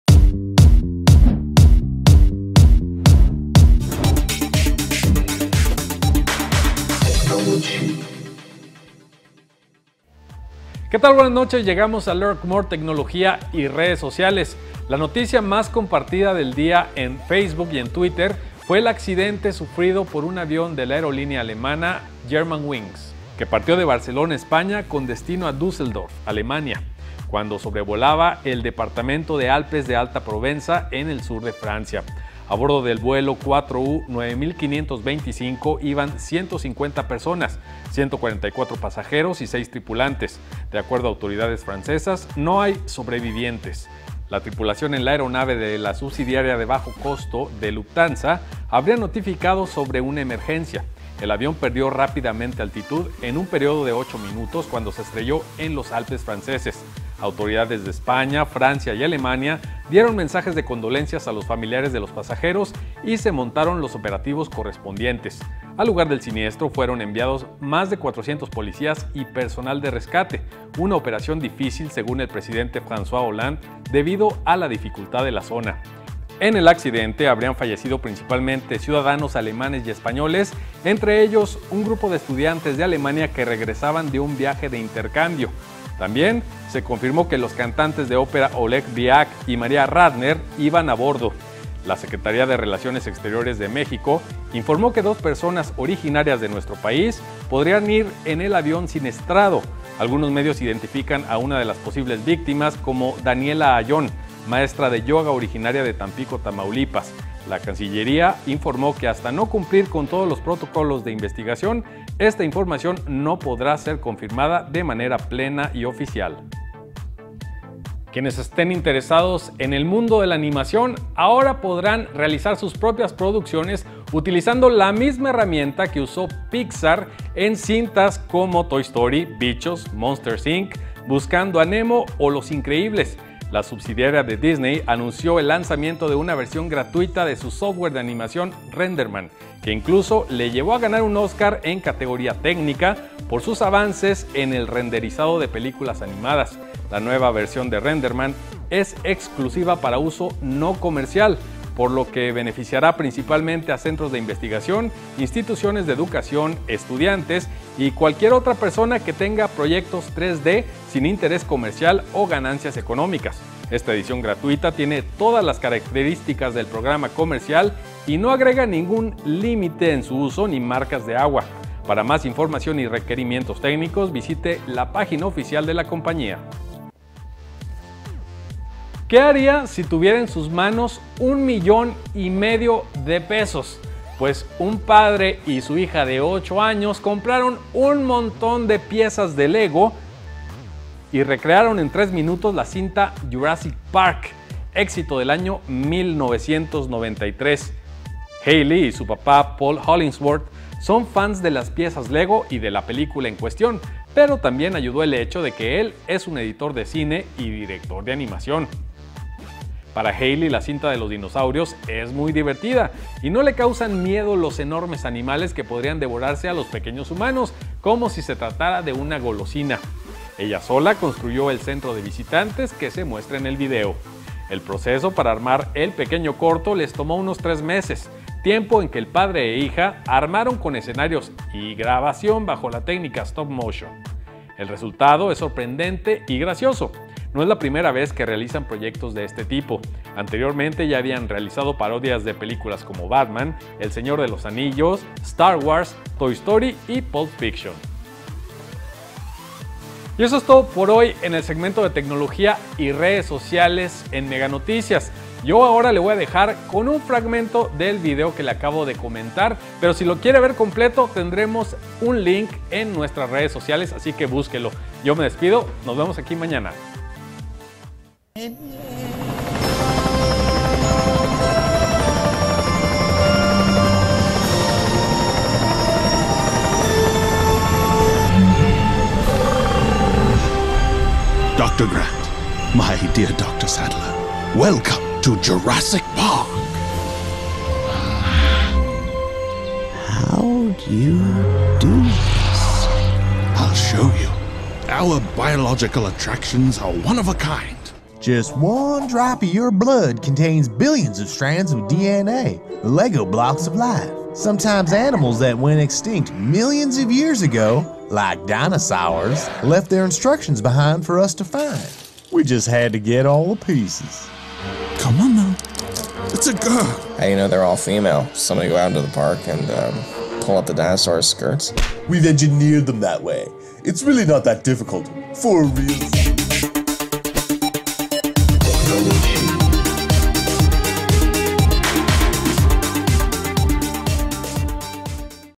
LM 240315 - Accidente de Germanwings, Pixar RenderMan, Recrean Jurassic Park con Legos
Transmite de lunes a viernes 8.30 de la noche por Megacanal Laguna. y

TEMAS DEL DÍA:

Noticias Virales

La noticia más compartida del día en las redes sociales fue el accidente sufrido por un avión de la aerolínea Germanwings que partió de Barcelona España, con destino a Düsseldorf Alemania, cuando sobrevolaba el departamento de Alpes de Alta Provenza en el sur de Francia.  A bordo del vuelo 4U9525 iban 150 personas, 144 pasajeros y 6 tripulantes.  De acuerdo a las autoridades francesas, no hay sobrevivientes.
La tripulación aeronave de la subsidiaria de bajo costo de la compañía Lufthansa, habría notificado sobre una emergencia.  El avión perdió rápidamente altitud en un periodo de 8 minutos cuando se estrelló en los Alpes Franceses.  Autoridades de España, Francia y Alemania dieron mensajes de condolencias a los familiares de los pasajeros y se montaron operativos correspondientes.  Al lugar del siniestro fueron enviados más de 400 policías y personal de rescate, una operación difícil según el presidente François Hollande, debido a la dificultad de la zona.
En el accidente habrían fallecido principalmente ciudadanos alemanes y españoles, entre ellos un grupo de estudiantes de Alemania que regresaban de un viaje de intercambio en España.  También se confirmó que los cantantes de ópera Oleg Bryjak y Maria Radner también iban a bordo.  La Secretaría de Relaciones Exteriores de México informó que dos personas originarias de nuestro país podrían ir en el avión siniestrado.  Algunos medios identifican a una de las posibles víctimas como Daniela Ayón maestra de yoga originaria de Tampico Tamaulipas.

Virales

Quienes estén interesados en la animación ahora podrán usar la misma herramienta que el estudio Pixar utilizó para creaciones como Toy Story, Bichos, Monsters Inc, Buscando a Nemo o Los Increíbles. La subsidiaria de Disney anunció el lanzamiento de una versión gratuita de su software de animación RenderMan, que incluso le llevó a gana un Oscar en categoría técnia por sus avances en el renderizado de películas animadas.  La nueva versión de RenderMan es exclusiva para uso No Comercial, por lo que beneficiará principalmente a centros de investigación, instituciones de educación, estudiantes, y cualquier otra persona que tenga proyectos 3D sin interés comercial o ganancias económicas.  Esta edición gratuita tiene todas las características del programa comercial y no agrega ningún límite en su uso ni marcas de agua.  Para más información y requerimientos técnicos, visite la página oficial de la compañía.

Internet

 - Recrean Jurassic Park con bloques
¿Qué haría si tuviera en sus manos un millón y medio de pesos?  Un padre y su hija de 8 años gastaron casi el equivalente a esa cantidad para comprar piezas lego y recrear en 3 minutos la cinta “Jurassic Park” éxito del año 1993.  Hailee y su papá Paul Hollingsworth son fans de las piezas Lego y de la película en cuestión, pero también ayudó el hecho de que él es un editor de cine y director de animación.
Para Hailee, la cinta de los dinosaurios es muy divertida y no le causan miedo los enormes animales que podrían devorarse a los pequeños humanos como si se tratara de una golosina.  Ella sóla construyó el centro de visitantes que se muestra en el video.
El proceso para armar el pequeño corto, les tomó unos 3 meses.  Tiempo en que padre e hija armaron con escenarios y grabación bajo la técnica “Stop-Motion”. El resultado es sorprendente y gracioso.
No es la primera vez que realizan proyectos de este tipo.  Anteriormente ya habían realizado parodias de películas como Batman, El Señor de los Anillos, Star Wars, Toy Story y Pulp Fiction.

NOTA DE COPYRIGHT:  Videos, imágenes y logotipos pertenecen a sus respectivos dueños.  Son utilizadas en esta pieza únicamente con carácter informativo.